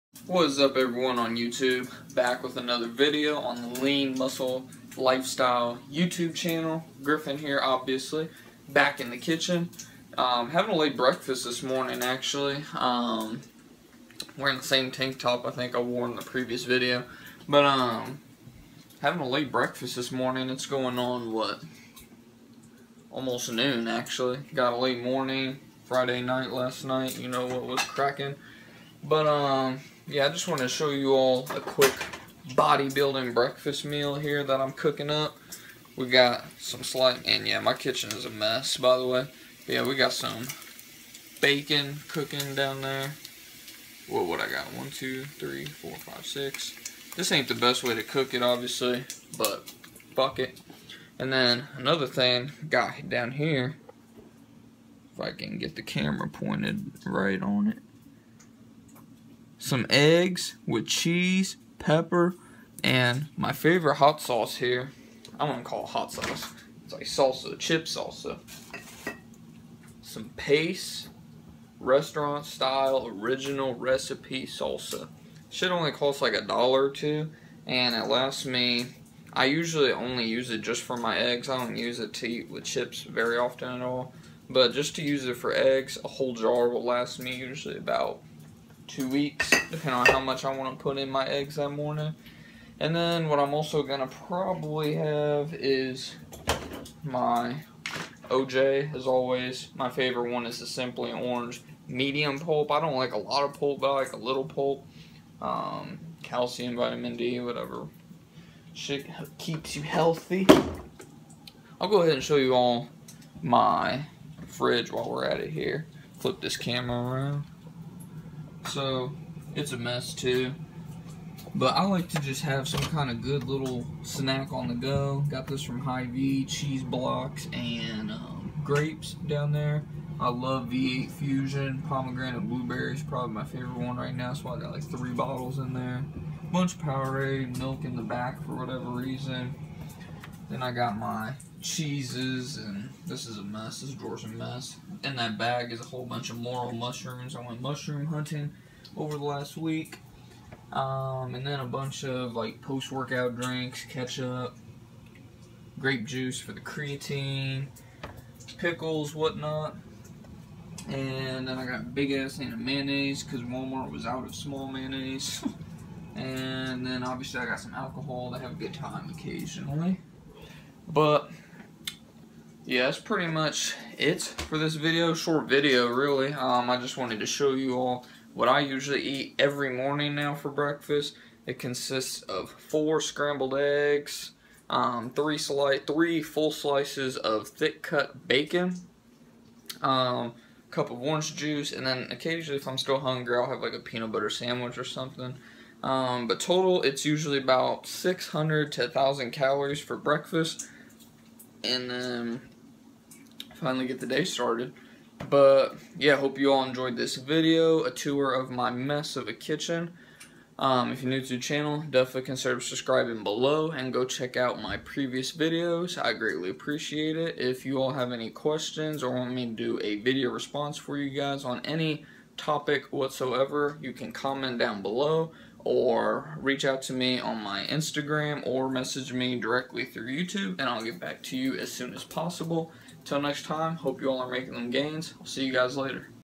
Bodybuilding Breakfast Meal!
Today we are back in the kitchen! Showing you guys how easy it is to get a good nutrient bodybuilding meal in first thing in the morning so you can make some more gains!

Have a question you'd like a video response over? Message me and I'll get to it asap!